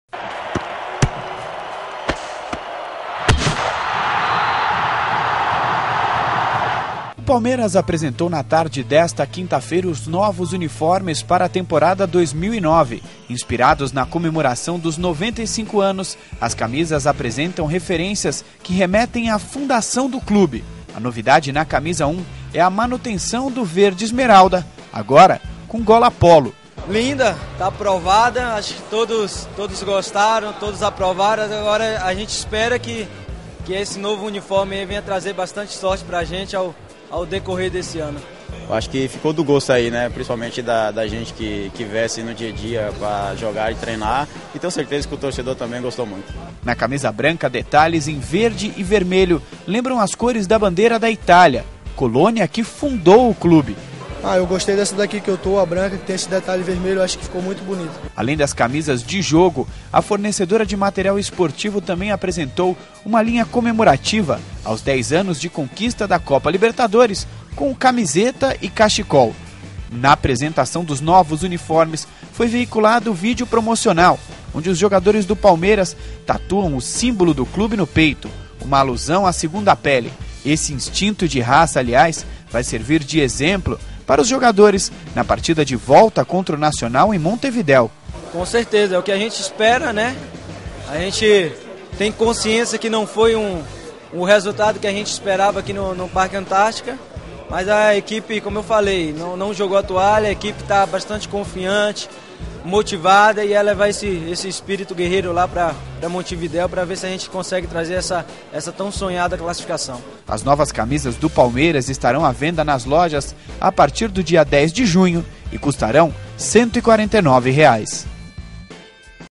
Palmeiras lança uniforme 09/10
Uol Esportes

Inspirado em sua fundação, Palmeiras lança novos uniformes